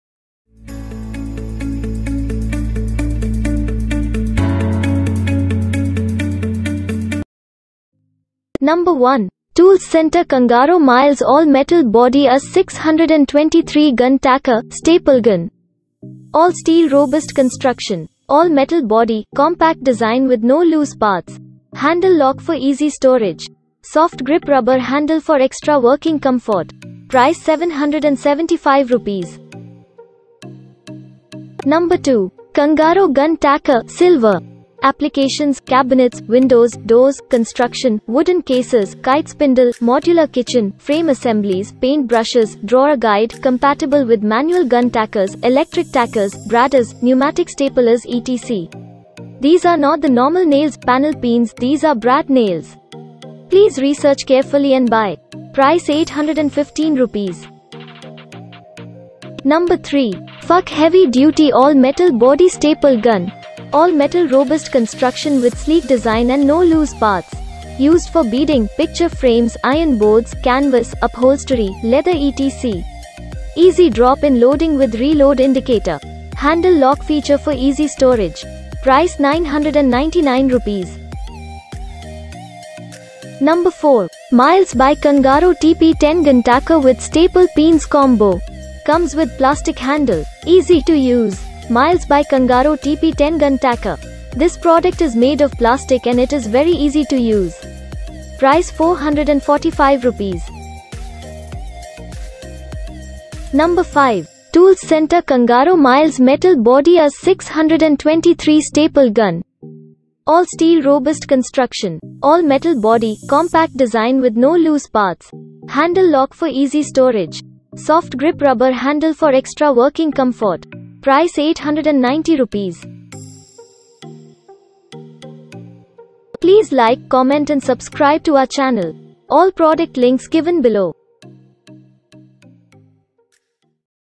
5 Best Staple Guns Available in India
► ► Links to Staple Guns we listed in this video:

► 1. Toolscentre Kangaro Miles All Metal Body Staplegun
► 2. Kangaro Gun Tacker
► 3. FAC Heavy Duty All Metal Body Staple Gun
► 4. Miles By Kangaro Tp-10 Guntacker
► 5. Toolscentre Kangaro Miles Metal Body Staple Gun